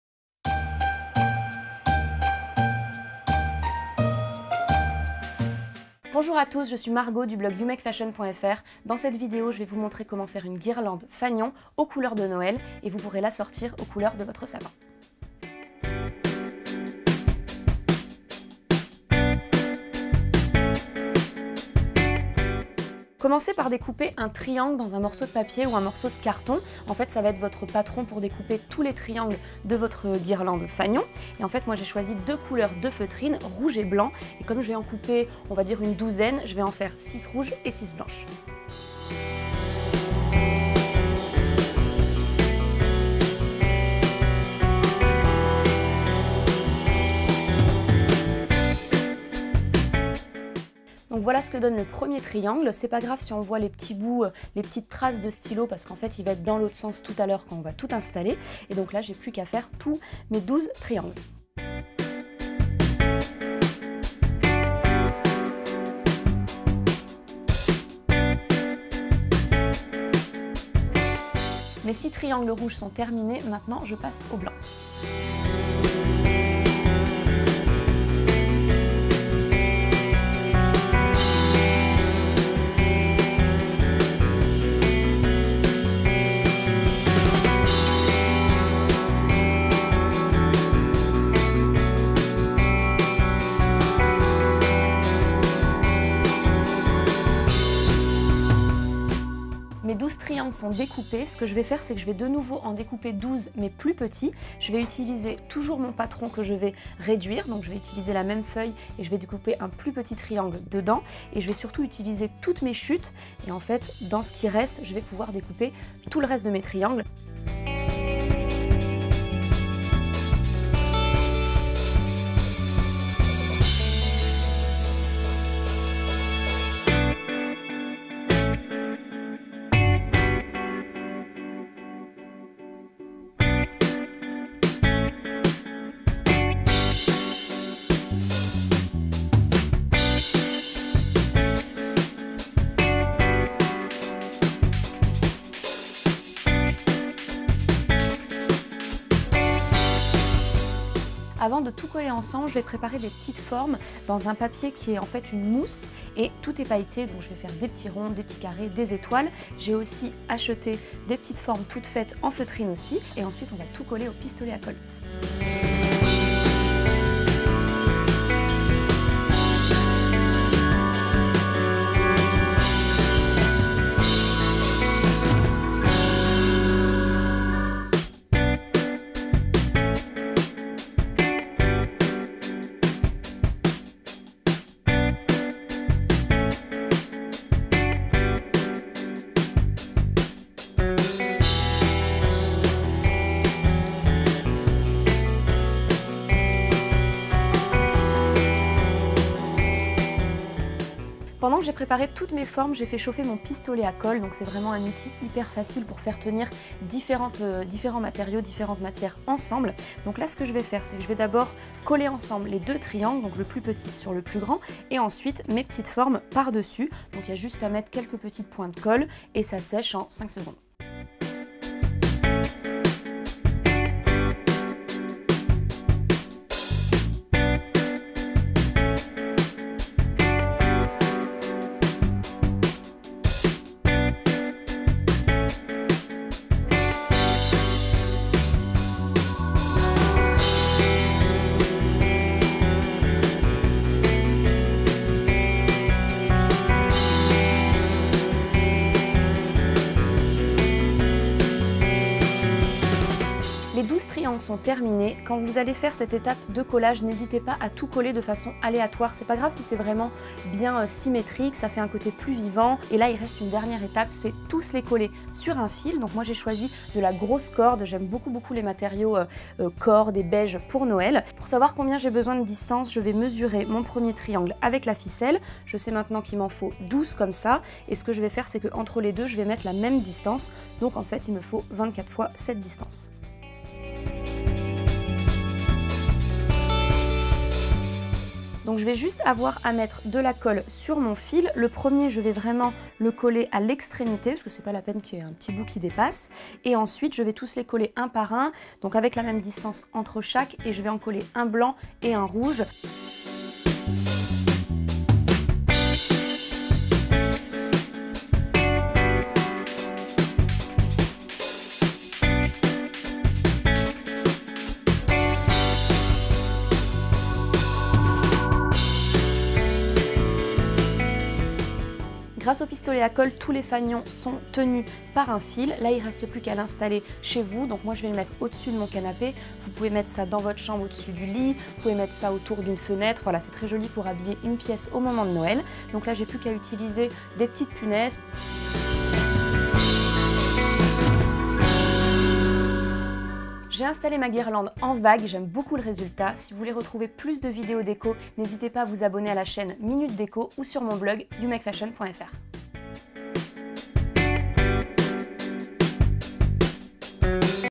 Fabriquer une guirlande de fanions aux couleurs de Noël - DIY avec youMAKEfashion
Margot vous montre comment fabriquer une guirlande de fanions pour Noël.

Découpez un triangle dans un morceau de carton, il constituera votre patron pour découper tous les triangles de votre guirlande.
Découpez les triangles : 6 rouges et 6 blancs.

Découpez-en 12 plus petits, pour cela utilisez les chutes de votre dernière découpe.

Avec un pistolet à colle, collez ensemble les deux triangles (le plus petit sur le plus grand).

Collez les triangles sur votre fil. Alternez les blancs et les rouges.
Tous les fanions sont désormais tenus par un fil.